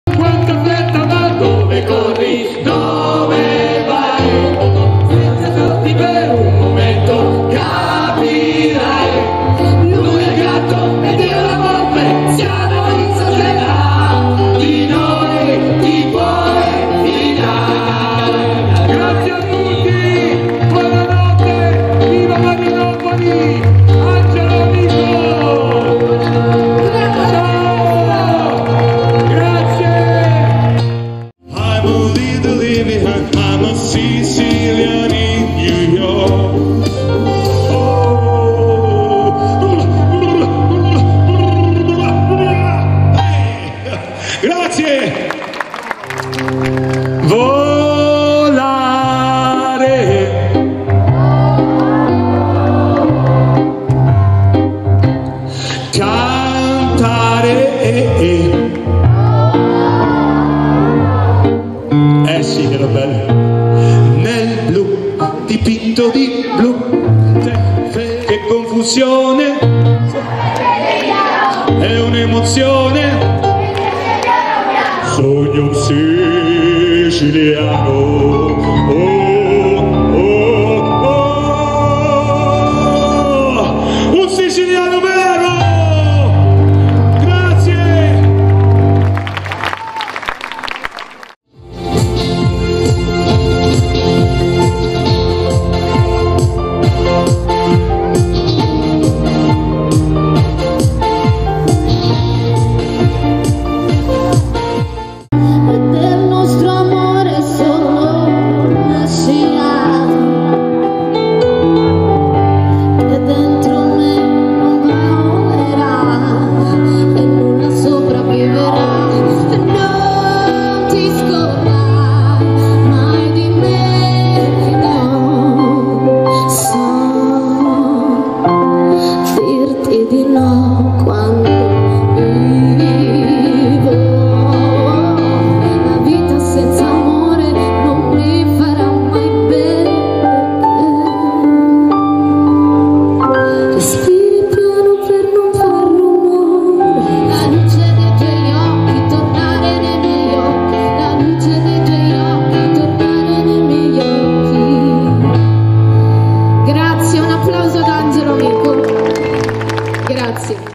Highlights Summer Show 2022 (Angelo Amico)
Spettacolo musicale, comico e di varietà con Ernesto Trapanese, Ivan Fiore, Simone Riccobono, Mago Plip, Cristina Ghanji, Cristian Abbate, Vincenzo La Mantia

PROFILI SOCIAL
LINKTREE